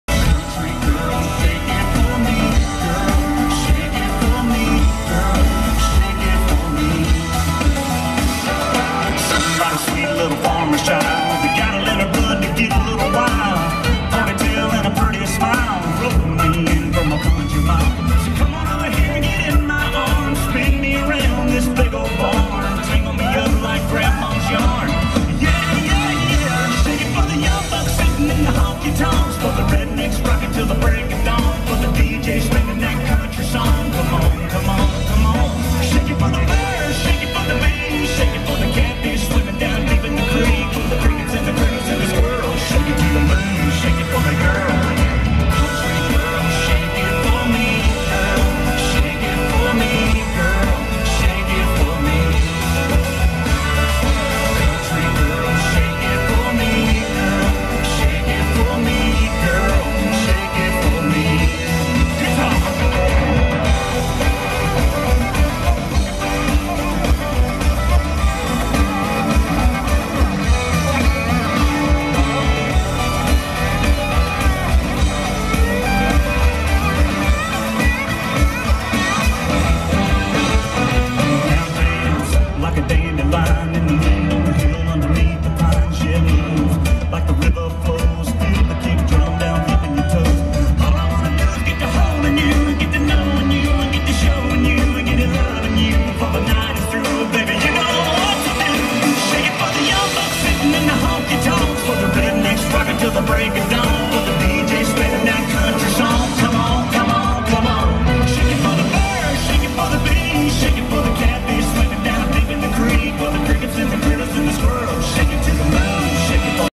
Fremont Street Experience -- Dancing DJ #10
A dancing DJ at the Fremont Street Experience.  December 16, 2015.  During my 12th trip to Las Vegas.